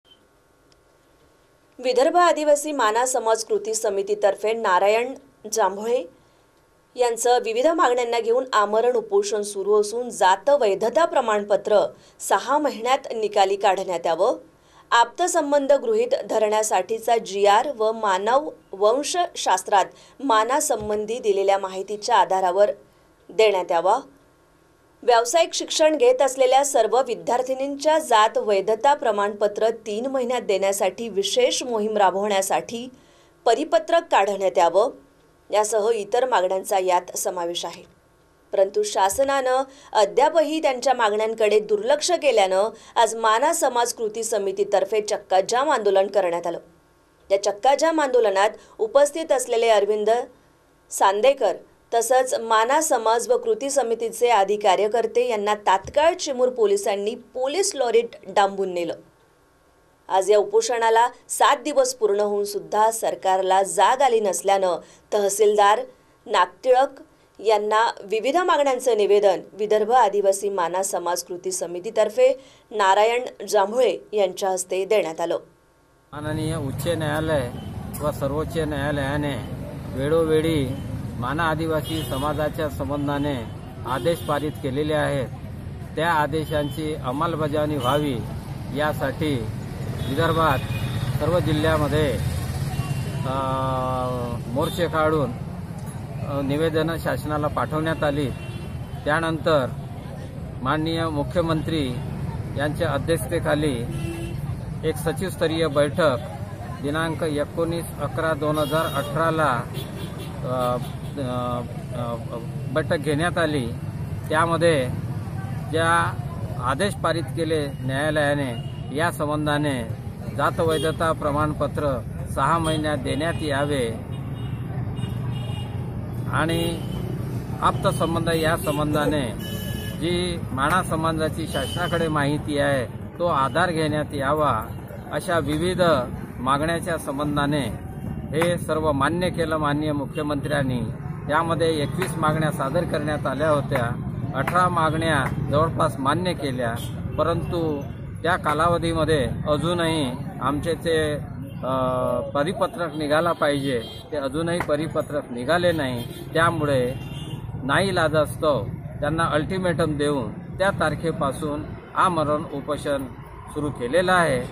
5 March 2019 Search Tv News Chandrapur - चिमूर - माना समाज कृती समिती तर्फे
चिमूर - माना समाज कृती समिती तर्फे करण्यात आले चक्का जाम आंदोलन